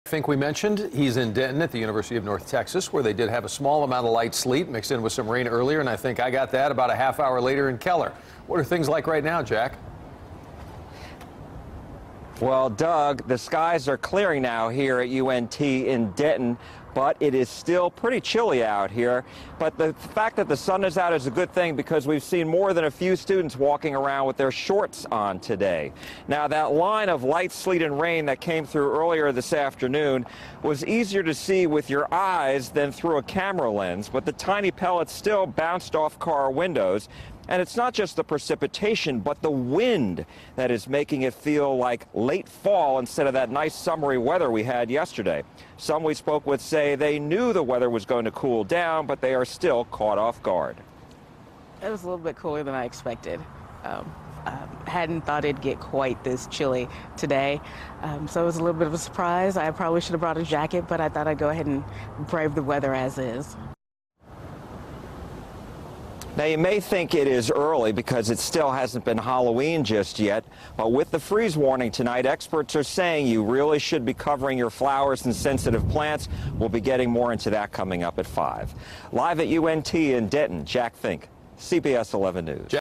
Sleet And Rain Greets North Texas Friday
Students in shorts at UNT braving the chilly temperatures -- and others bundled up -- wearing hoodies, and even covering their mouths.